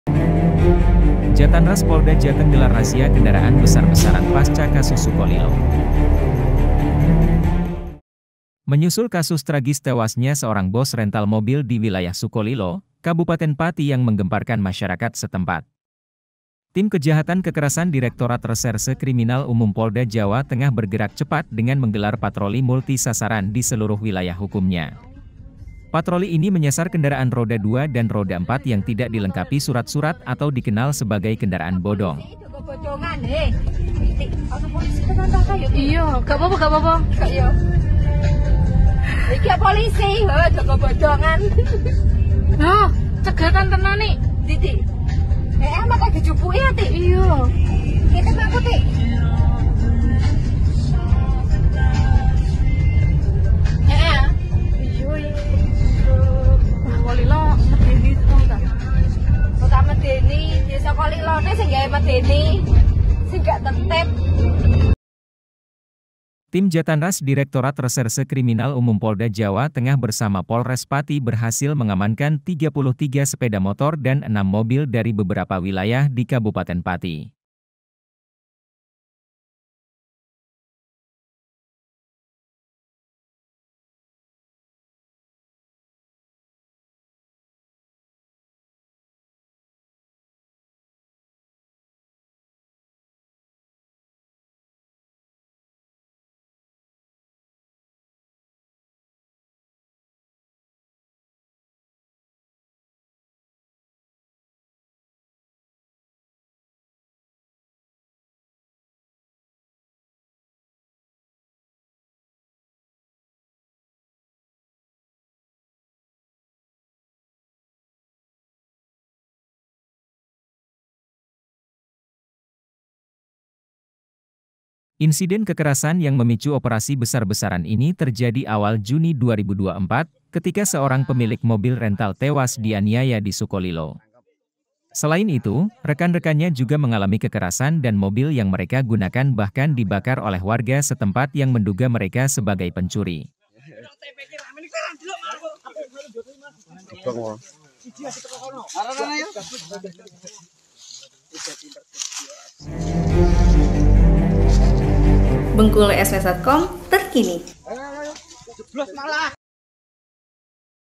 Jatanras Polda Jateng Gelar Razia Kendaraan Besar besaran Pasca Kasus Sukolilo
Menyusul kasus tragis tewasnya seorang bos rental mobil di wilayah Sukolilo, Kabupaten Pati yang menggemparkan masyarakat setempat, Tim Kejahatan Kekerasan Direktorat Reserse Kriminal Umum Polda Jawa Tengah bergerak cepat dengan menggelar patroli multi sasaran di seluruh wilayah hukumnya.

Patroli ini menyasar kendaraan roda dua dan roda empat yang tidak dilengkapi surat-surat atau dikenal sebagai kendaraan bodong.

Tim Kejahatan Kekerasan (Jatanras) Direktorat Reserse Kriminal Umum Polda Jawa Tengah (Jateng) bersama Polres Pati berhasil mengamankan 33 sepeda motor dan 6 mobil dari beberapa wilayah di Kabupaten Pati .

Kepala Bidang Humas Polda Jateng, Kombes Pol Stefanus Satake Bayu Setianto, mengonfirmasi bahwa kendaraan-kendaraan tersebut diamankan karena dokumennya tidak lengkap.

"Betul, anggota masih di sana. Kendaraan dokumennya tidak lengkap," ujarnya kepada wartawan pada Kamis (13/6/2024).

Insiden kekerasan yang memicu operasi besar-besaran ini terjadi awal Juni 2024, ketika seorang pemilik mobil rental tewas dianiaya di Sukolilo.

Selain itu, rekan-rekannya juga mengalami kekerasan dan mobil yang mereka gunakan bahkan dibakar oleh warga setempat yang menduga mereka sebagai pencuri.

Kombes Stefanus tidak dapat memberikan rincian lebih lanjut terkait anggota yang terlibat dalam pengamanan puluhan sepeda motor dan mobil tersebut.

Sementara itu Direktur Reserse Kriminal Umum Polda Jateng, Kombes Pol Johanson Simamora, menyebutkan bahwa barang bukti diamankan dari beberapa lokasi termasuk Sukolilo, Trangkil, dan Tambakromo.

"Ada tiga orang yang kami amankan, masih dalam pemeriksaan lebih lanjut," tambahnya.